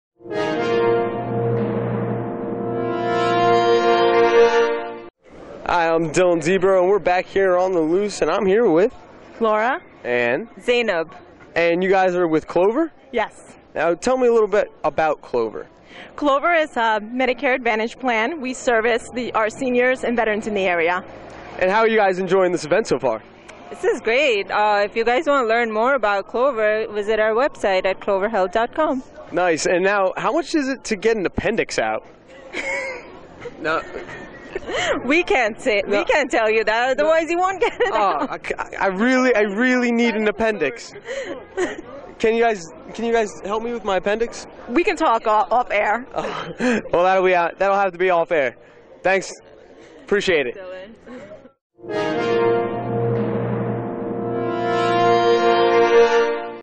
Laura of Clover Health at Military Appreciation Day , Manville Air Port
Laura at Military Appreciation Day,  Manville Air Port. Giving out information from Clover Health. Check them out !!!